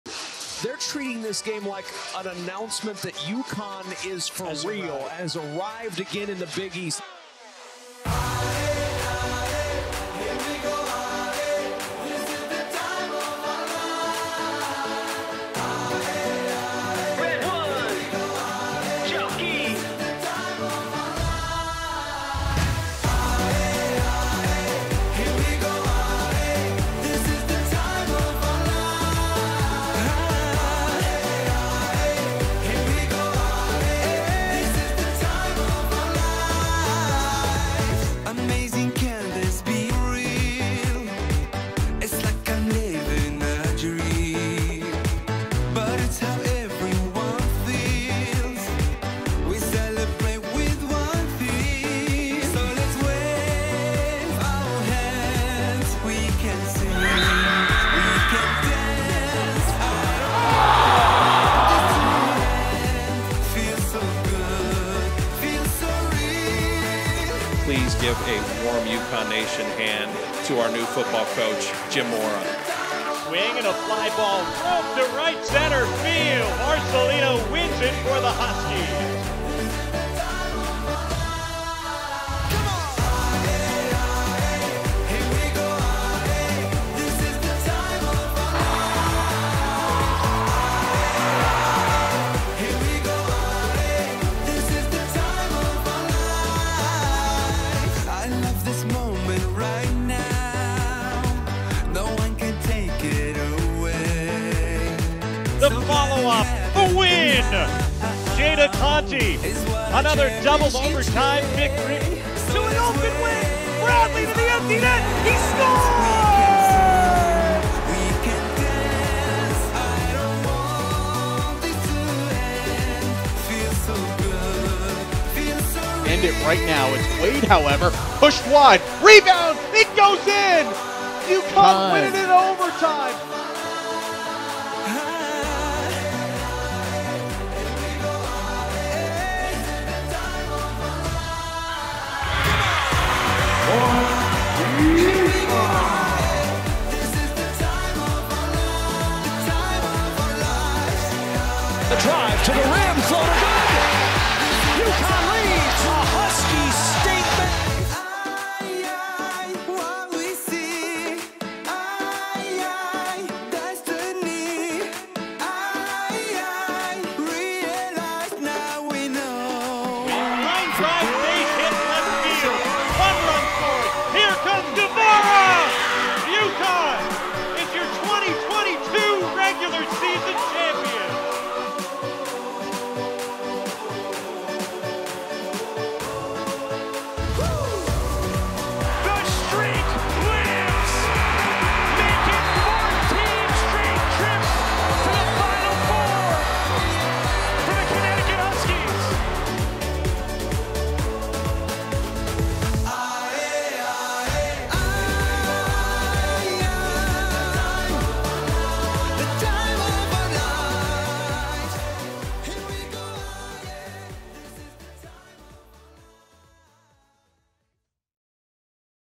UConn Huskies 2021-22 All-Sports Highlight Reel
Take a look back at some of the greatest moments from the 2021-22 season. What was your favorite?